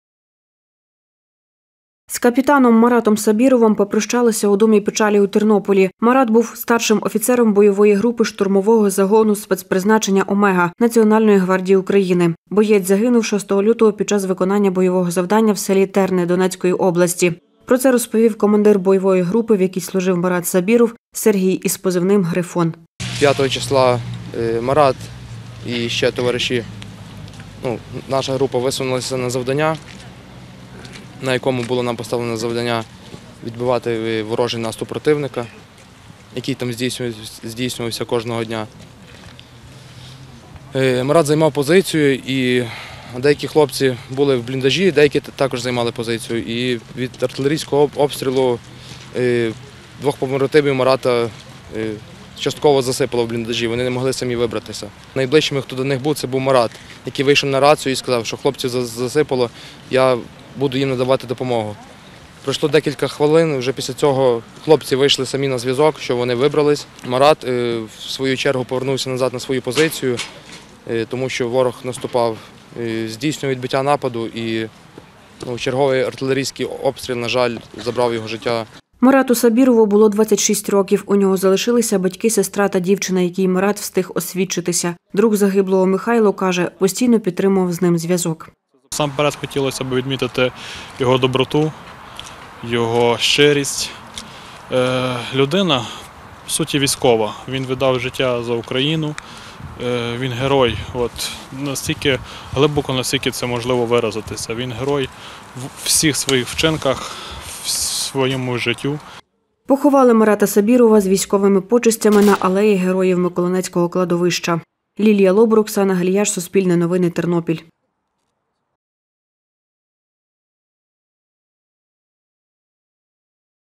«Він – герой у всіх своїх вчинках»: у Тернополі попрощалися із Маратом Сабіровим
У Тернополі попрощалися зі старшим oфіцером бoйoвoї групи штурмoвoгo загoну спецпризначення «Oмега» Нацгвардії України капітаном Маратом Сабірoвим. Боєць родом з Енергодара Запорізької області. Був кадровим військовим, закінчив Харківську академію Національної гвардії. За розподілом потрапив на службу до Тернополя, а тоді – на Донецький напрямок, де й загинув.